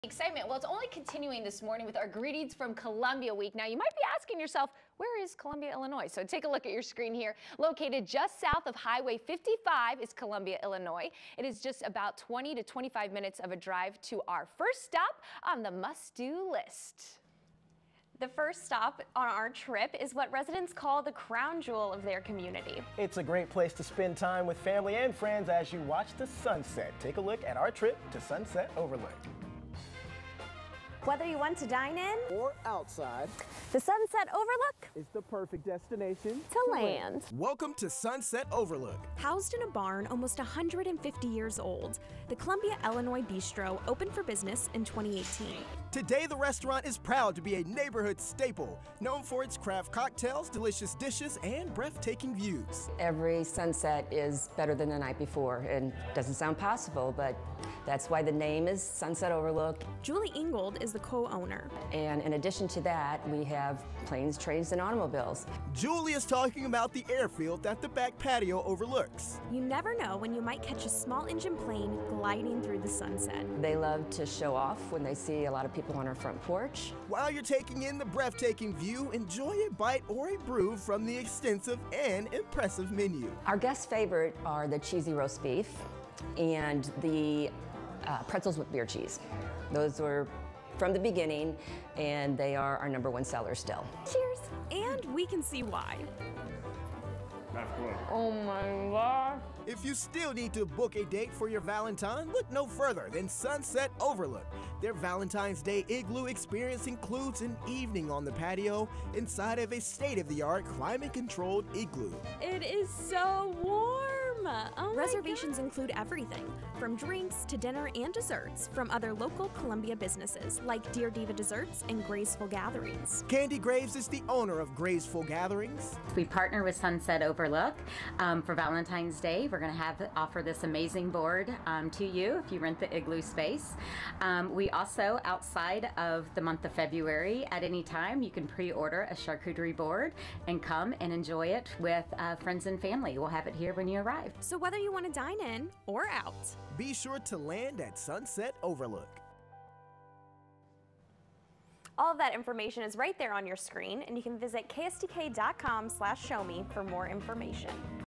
Sponsored: Sunset Overlook is a perfect landing spot for craft beer and good eats in Columbia, Illin
Sunset Overlook is a destination restaurant with incredible views and is considered the crown jewel of Columbia, Illinois.
SHOW ME ST. LOUIS IS A PART OF 5 ON YOUR SIDE AND FEATURES ST. LOUIS EVENTS, COMPANIES, BUSINESS PEOPLE AND OTHER GUESTS FROM AROUND THE COUNTRY.